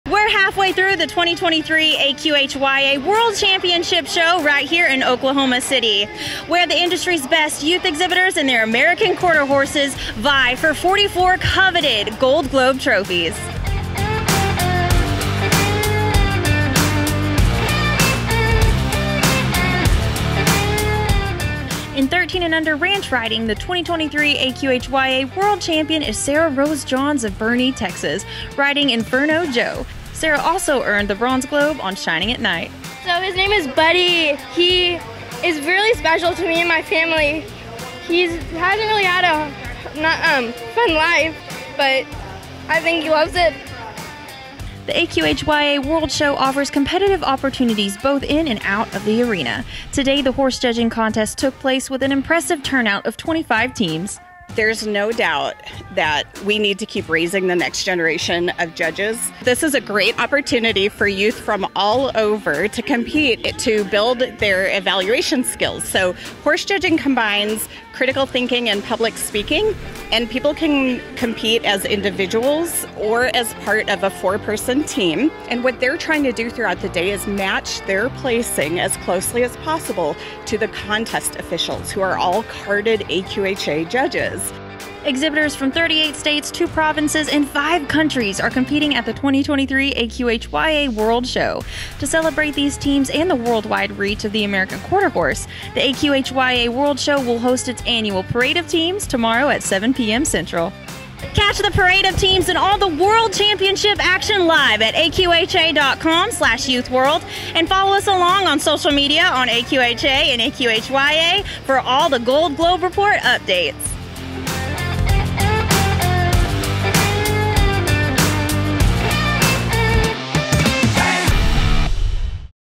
AQHYA World Championship Show Gold Globe Report: August 1, 2023
Meet the ranch riding 13-&-under world champion, plus highlights from the horse judging contest and Parade of Teams in tonight's Gold Globe Report, presented by Nutrena Feed.